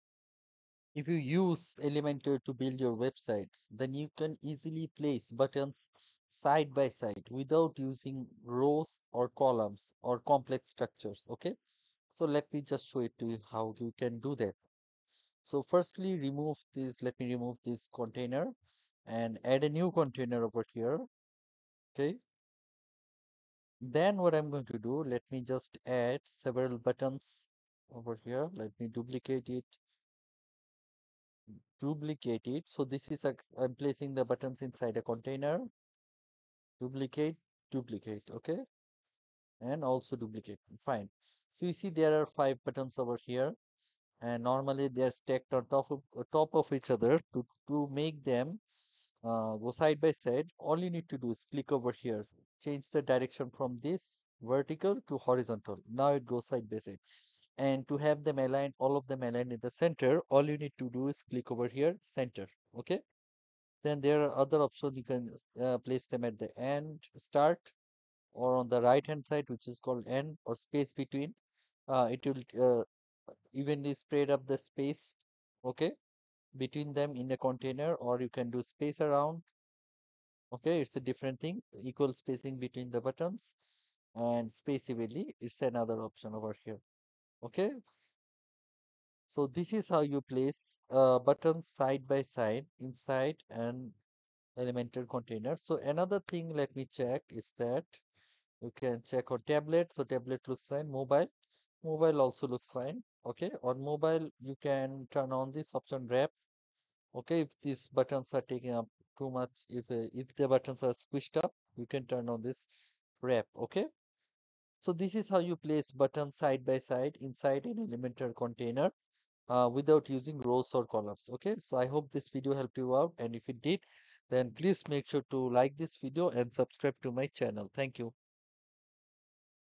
How to place buttons side by side in Elementor Flexbox container on WordPress 2024
In this Elementor tutorial video, I am going to show how to place Elementor buttons next to each other, so they are Horizontally Aligned Buttons in a single row, within an Elementor flexbox container.

Simply by setting the Elementor flexbox container setting to row, we can place the buttons side by side. This will make the buttons horizontally aligned.

Need support or help with Elementor Wordpress? Then view my gig on Fiverr: